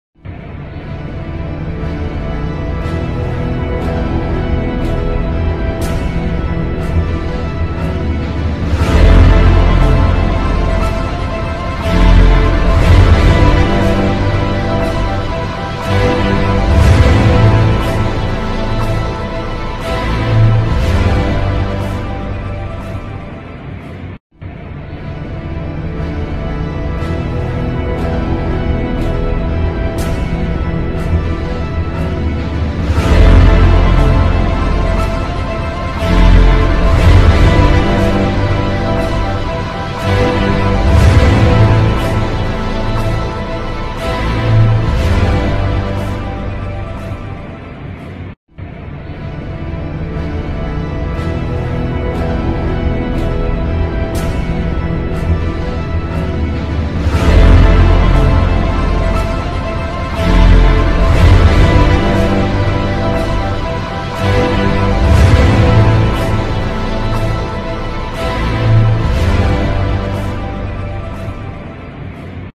When Your First Day At The Car Wash Goes Wrong!
Man is tied up and spinnend around inside the carwash while cleaning up!